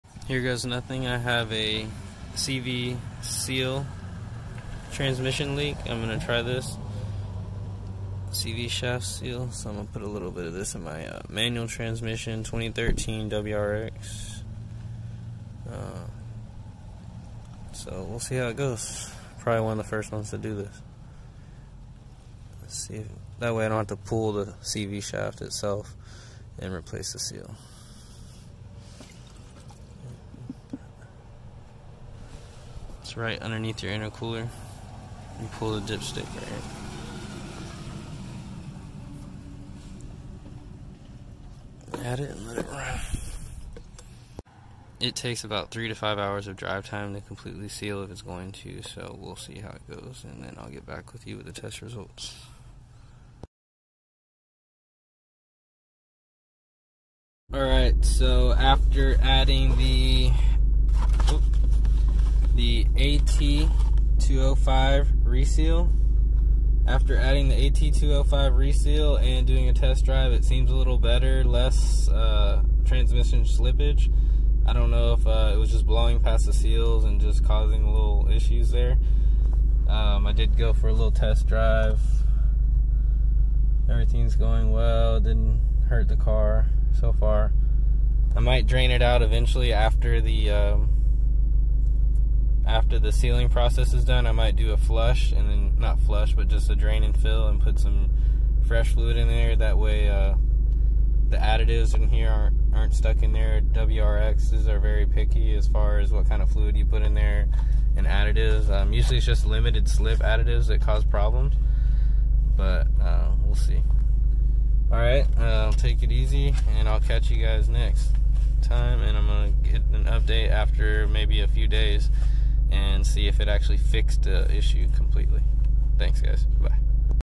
AT-205 Re-seal on Subaru WRX Manual Transmission Leak
Testing AT 205 Reseal on leaking CV shaft seal.
2013 Subaru WRX 5 Speed Manual Transmission. Test drive was great felt a gradual improvement I think.. to be continued.